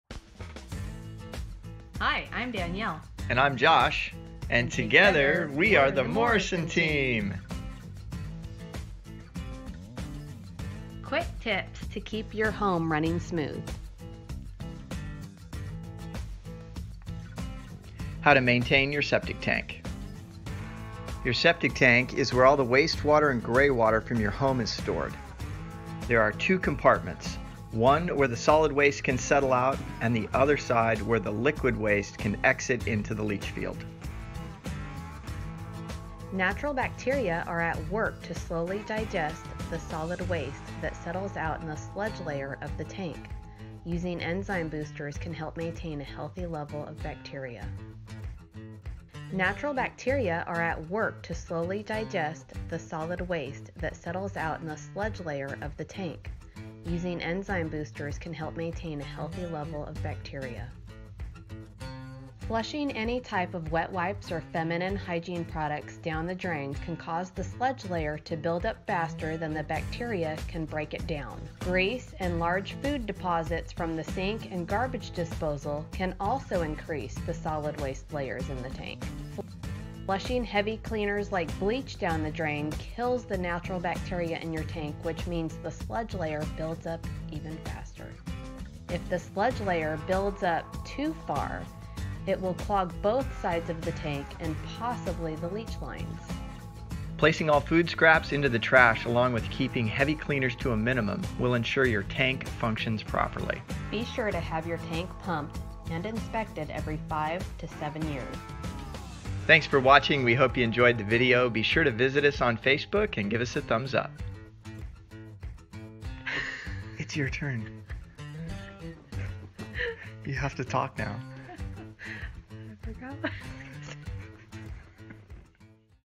Septic Tank Info
A quick animated video on how to maintain your septic tank